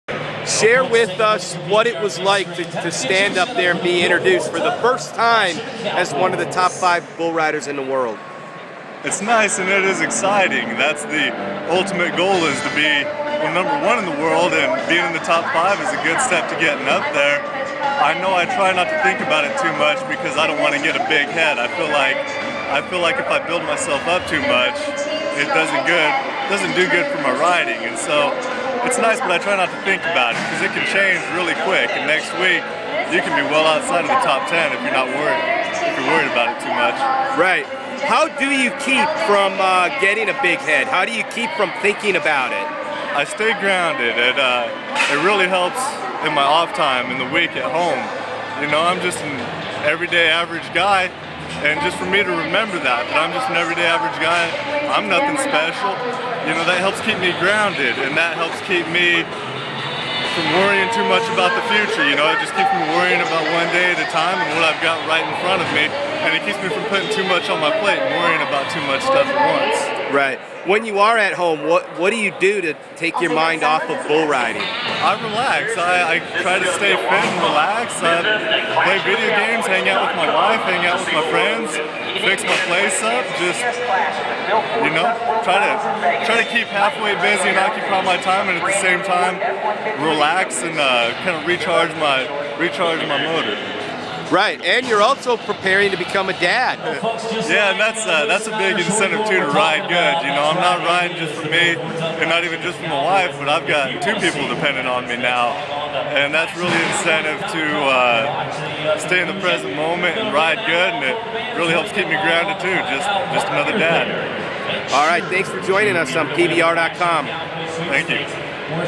Jordan Hupp: 'I stay grounded' (PBR)
Keith Ryan Cartwright talks with Jordan Hupp about being ranked in the Top 5, as well as what he does during the week to stay grounded.